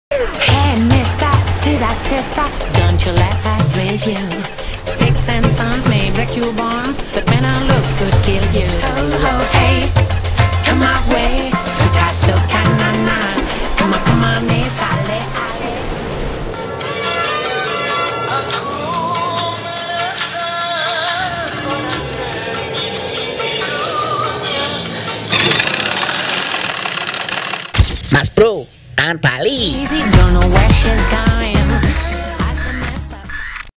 Bali Paradise I Miss You
Mas Bro means Indonesian slank nick name same as "Bro or Brother" (Es Beantoro), have been travel to Bali and he always remember the best moments in Bali. Unforgetable. "Mas Bro Kangen Bali" means "Mas Bro Miss Bali so much". Part 2.